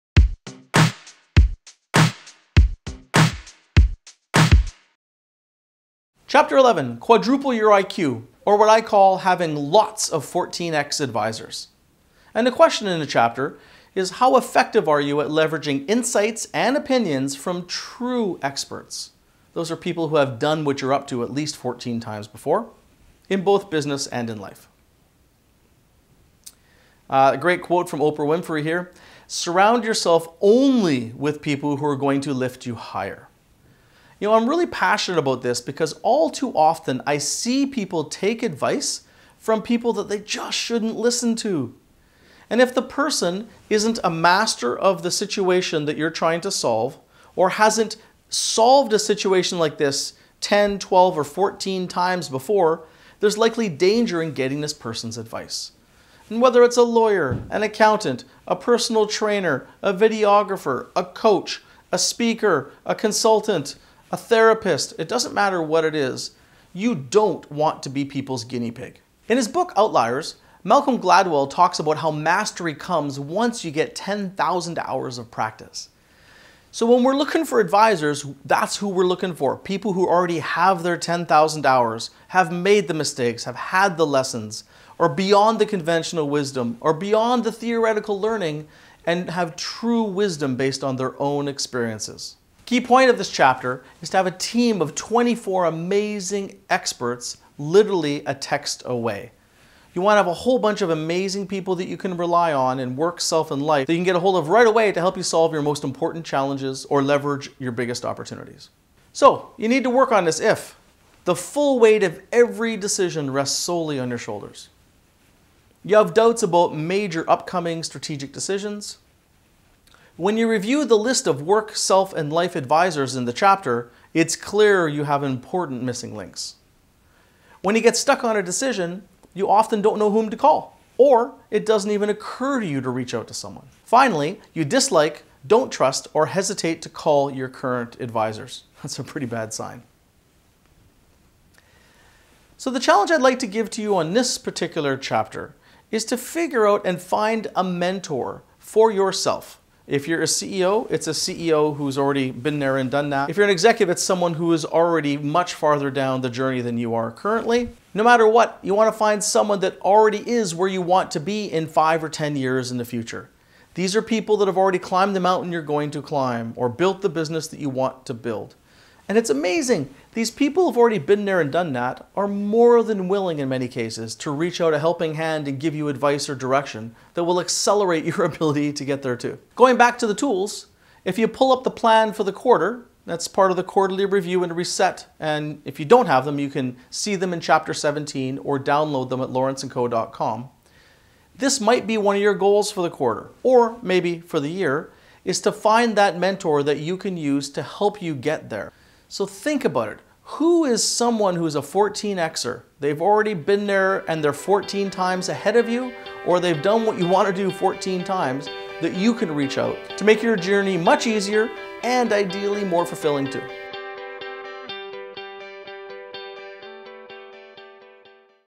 Chapter 11 Quadruple Your IQ
How effective are you at leveraging insights & opinions from true experts in business and in life? Learn about the value of trusted advisors to support your strategic and life decisions.